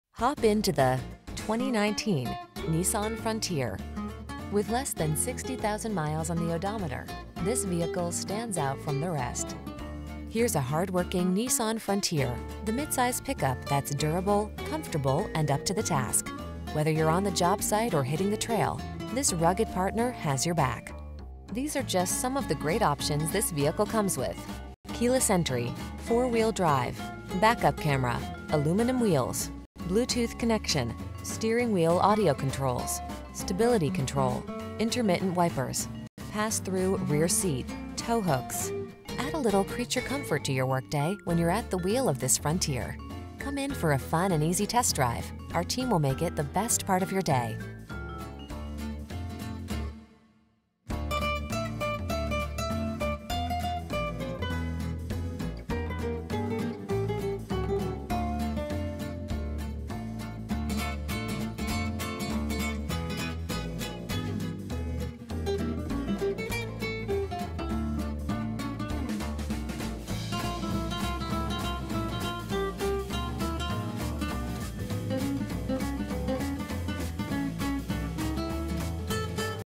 2019 Nissan Frontier St. Augustine, Palm Coast, Jacksonville, Daytona Beach, Gainesville, FL KN78989
Brilliant Silver Metallic Used 2019 Nissan Frontier available in St. Augustine, Florida at Nissan of St. Augustine. Servicing the Palm Coast, Jacksonville, Daytona Beach, Gainesville, FL area.